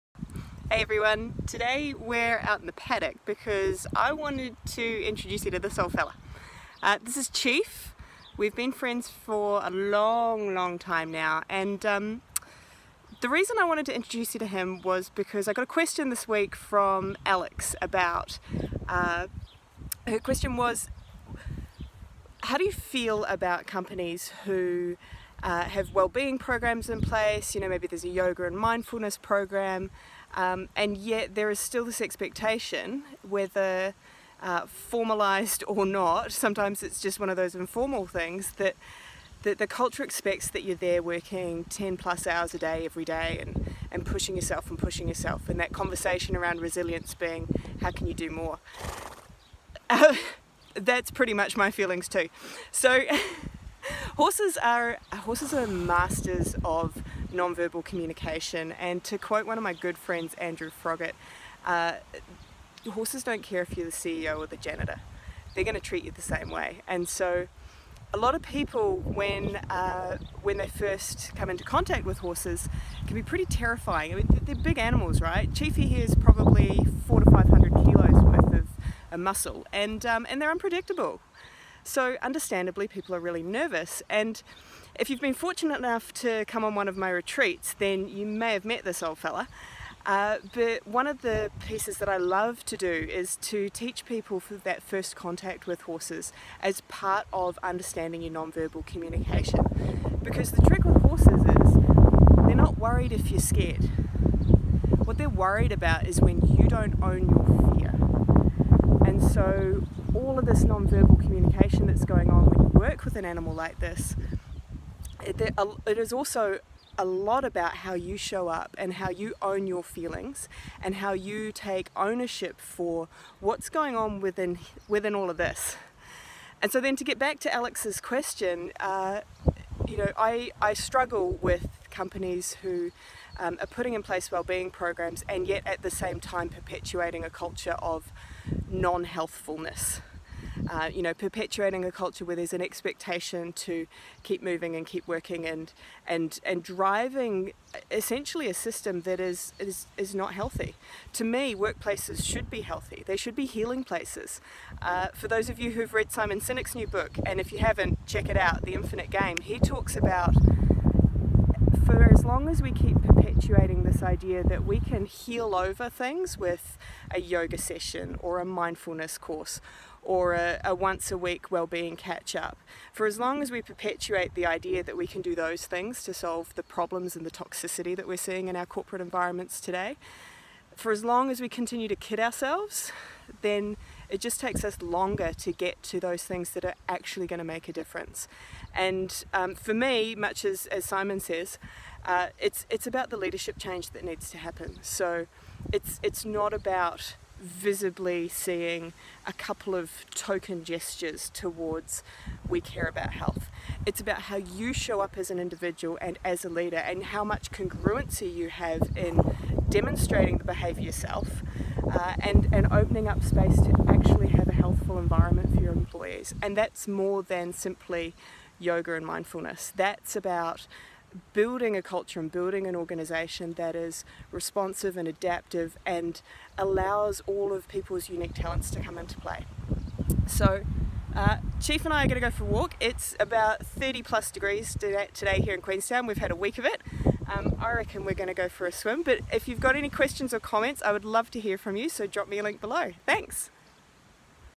Ditch the tokenism, build congruence
How do you feel about a company that has a wellbeing program yet perpetuates a culture of over-work?

Most of us find this type of tokenism leaves us hollow, cynical, disappointed.  And worse, these gestures without the depth of change to back them up keep us distracted from the real cause of the toxicity in our corporate environments.

But what's the alternative?  Have you seen it done well?

Well that's what this week's video is all about - and we have a cameo!